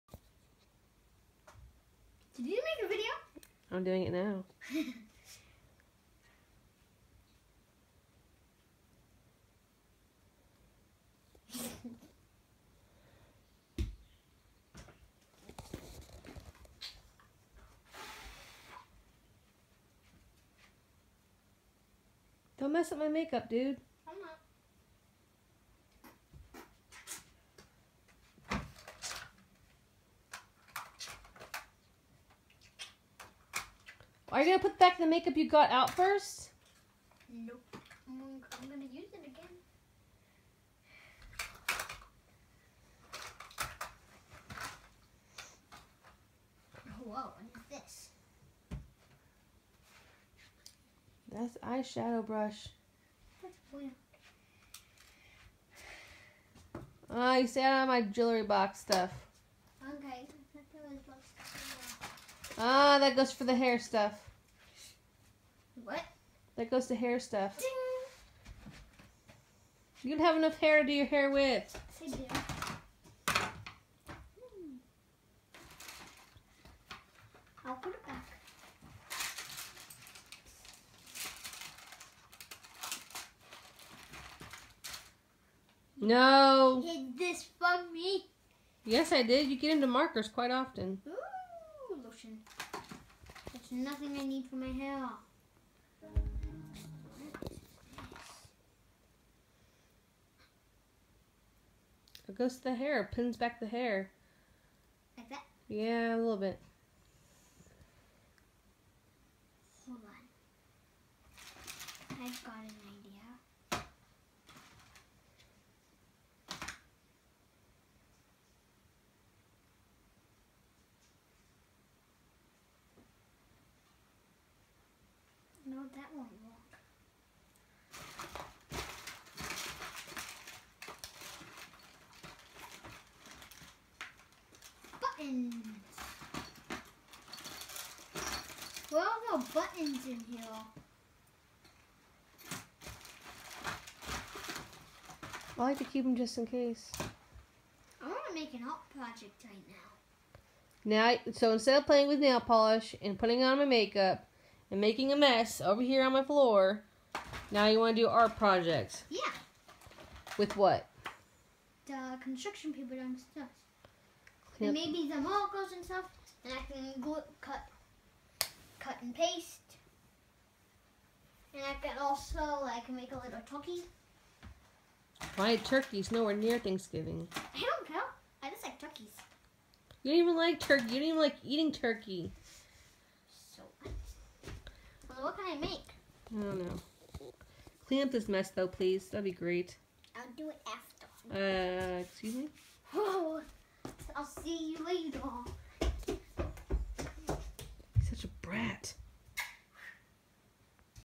Logan
Logan is being a goofball and playing in my makeup lol